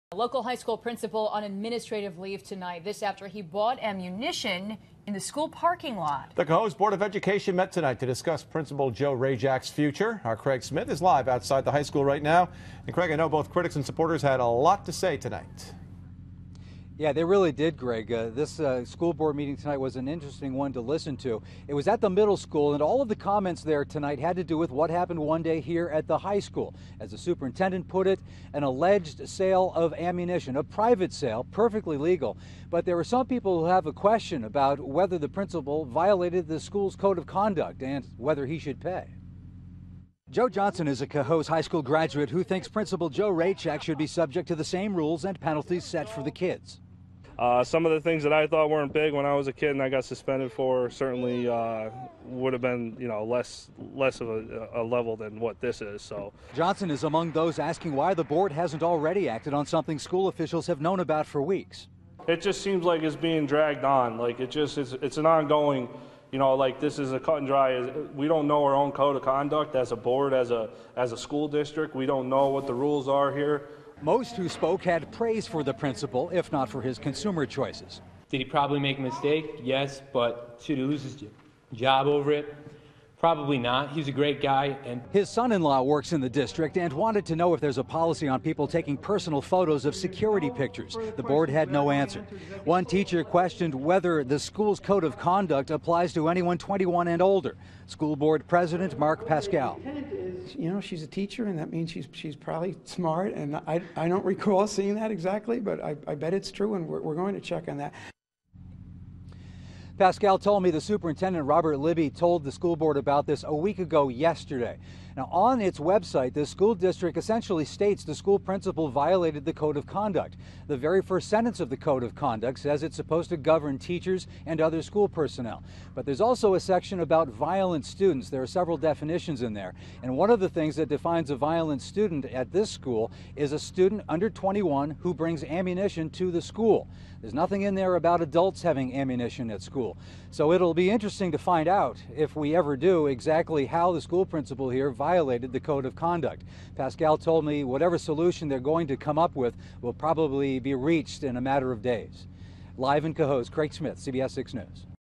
Cohoes Principal Put On Leave
The president of the school board in Cohoes says it will be a matter of days before there's a solution to the controversy over the high school principal buying ammunition in a private sale in the school parking lot.When the board met Thursday night at the middle school, it heard several comments from people who want to know details about the transaction and some who came to say Principal Joe Rajczak is a good man.After having opened the meeting in the middle school library, board President Mark Pascale thanked everyone for coming and the board went into executive session in a room on the next floor.The code of conduct at Cohoes High School states that it is to govern teachers and other school personnel in addition to students. It says any student under age 21 who has ammunition at school would be considered a violent student but says nothing about adults who possess ammunition. The school district's web site says the principal violated the code but does not say how.